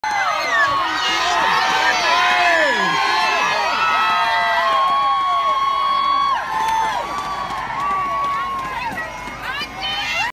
Andy Murray and best man Jamie arrive at Dunblane Cathedral for his wedding to Kim Sears
Both dressed in Murray tartan kilts, the brothers arrive to cheers from the 3,000 strong crowd at his wedding on April 11, 2015.